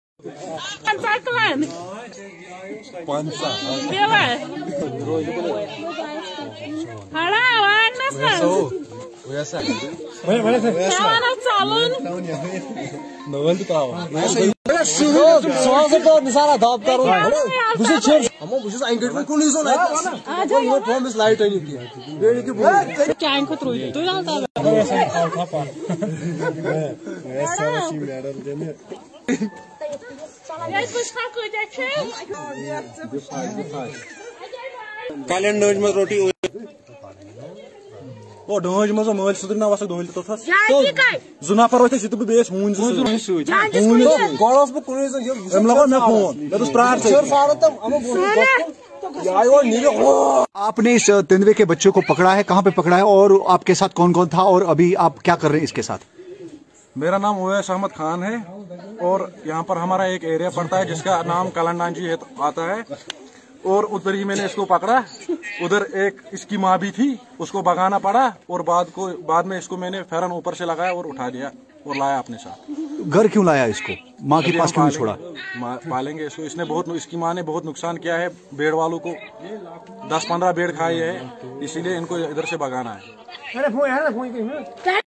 Cub to be reunited with mother after captured by a local in South Kashmir’s Anantnag
Anantnag, Apr 25, KNT: A baby leopard was captured in Southern district of Anantnag by a local claiming that its mother that escaped from the spot has savaged livestock in his village and villagers are keen to throw wild animals out from the area.

A purported video of a cub (baby leopard) is viral on social media. The cub surrounded by locals in the video was captured by one Owais Ahmed Khan.

 “I captured this cub near a place called Kalanji. Her mother was accompanying her. I forced her to flee and draped the cub in a ‘pheran’. Her mother has devoured dozens of live stock and she ought to be thrown out from the village,” Khan said.

One of the officials from Wild Life Achabal told news agency Kashmir News Trust that they have already taken the custody of the cub from locals in Kehribal area of Anantnag and efforts are on to reunite it with her mother. He said locals can’t be blamed for the act as big cats are attacking and killing their animals. (KNT)